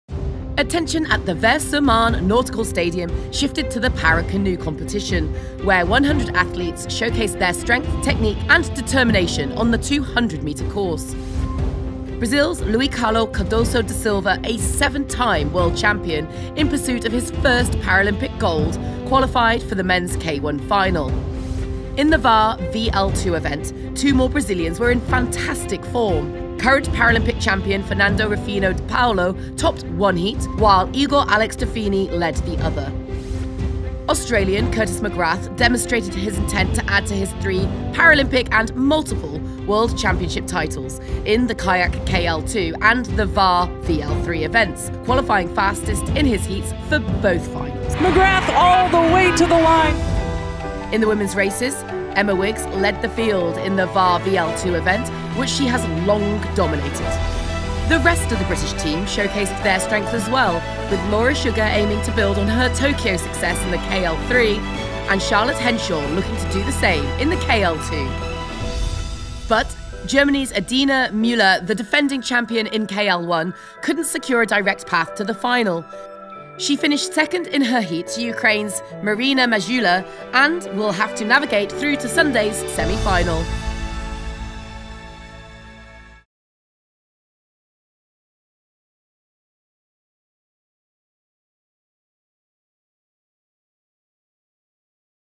🛶 Para Canoe Highlights | Day 9 | Paris 2024 Paralympic Games
Watch the highlights of Para Canoe day 9 of the Paris 2024 Paralympic Games ❤️💙💚